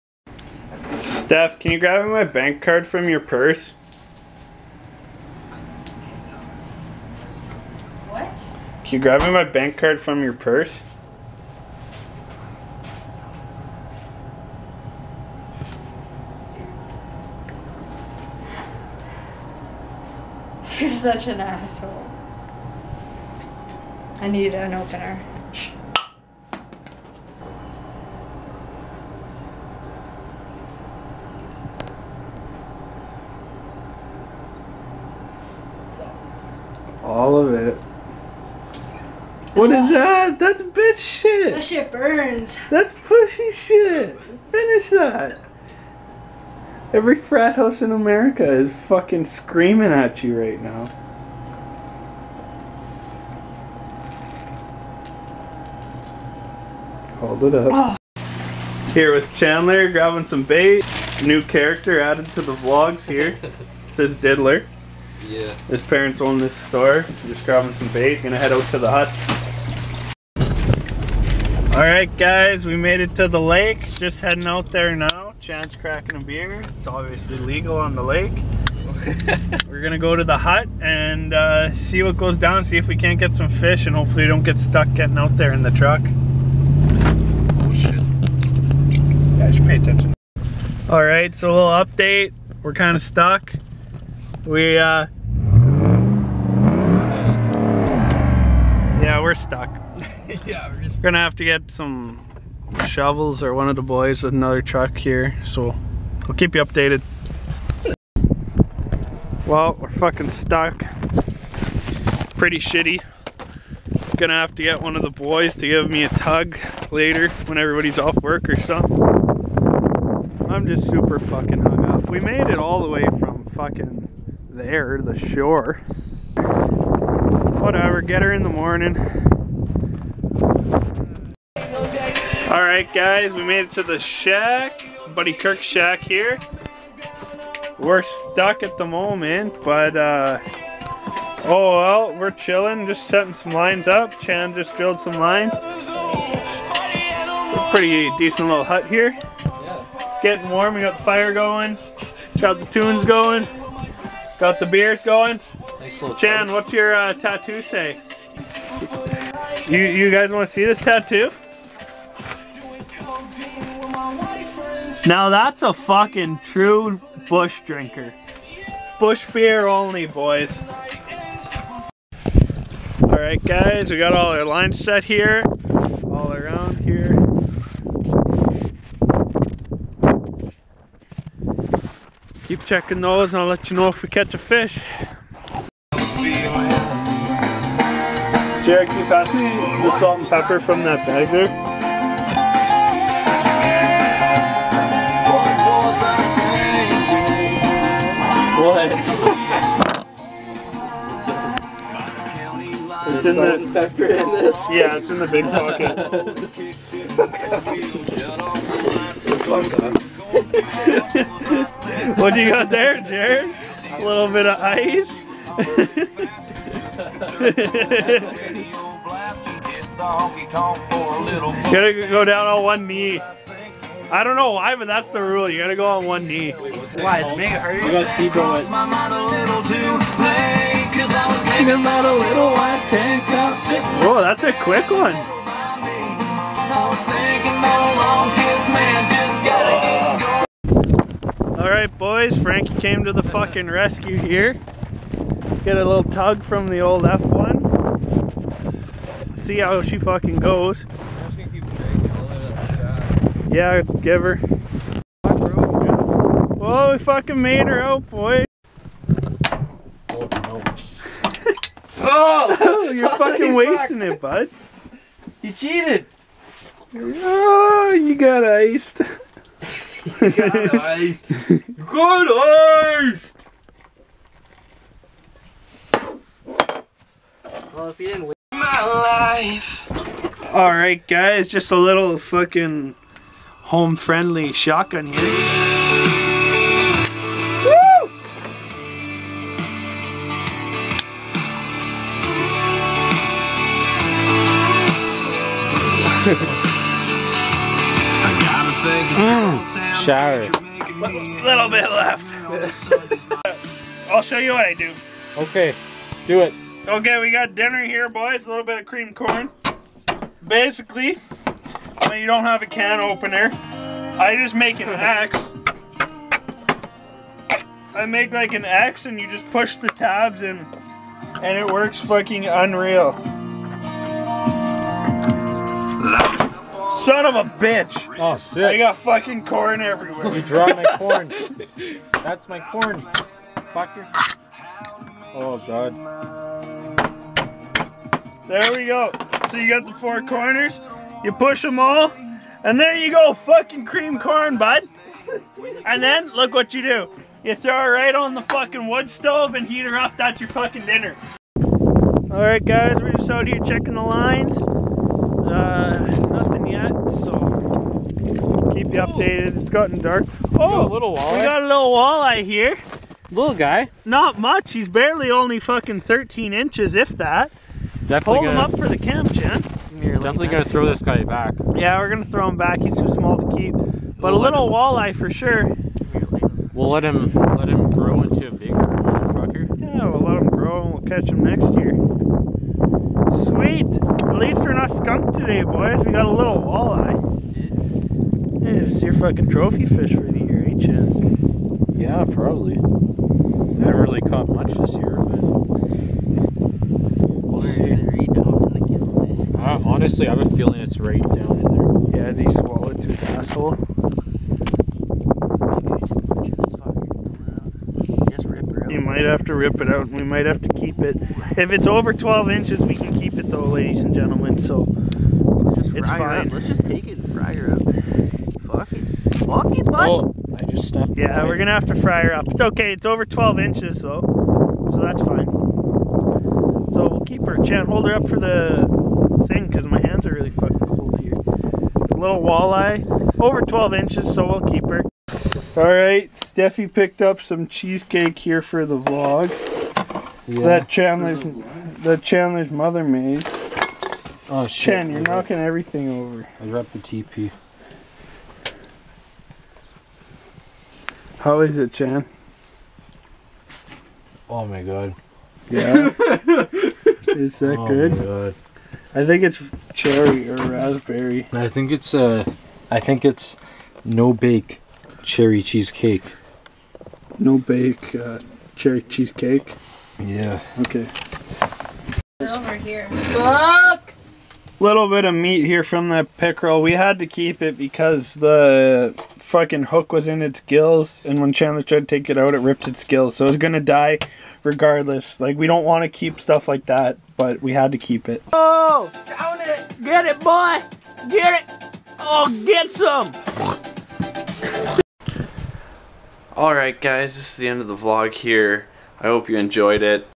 Vlog #2 getting stuck on the lake
we go fishing and get the truck stuck, catch a fish and get sauced.

i dont own or have copywrights for any of the back ground music.
anyone drinking is leagal drinking age.